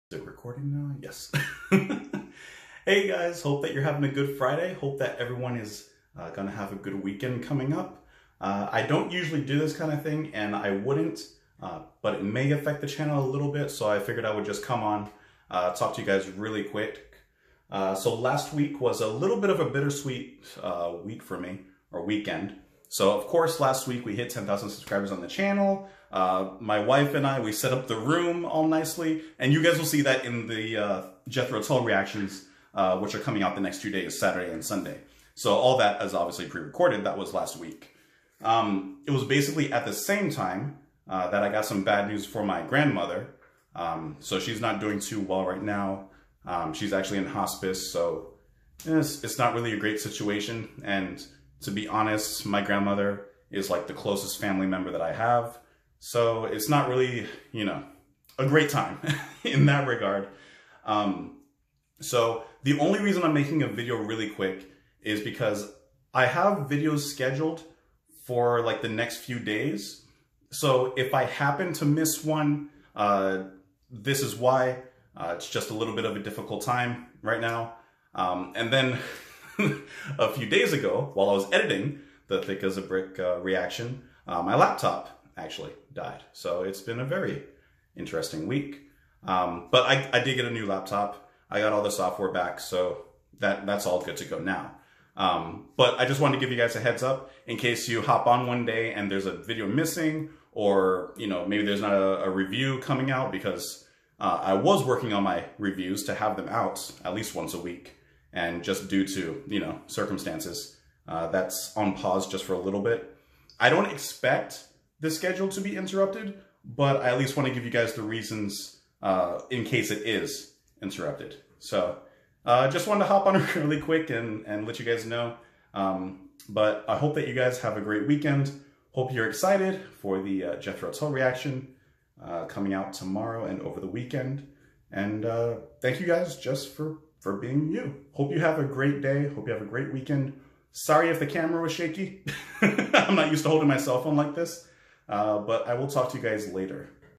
Some Quick News
TL;DW: family emergency, videos may not be daily for a little bit depending on situation. Its more likely than not that everything will be normal on the channel, but just in case not you know why🙃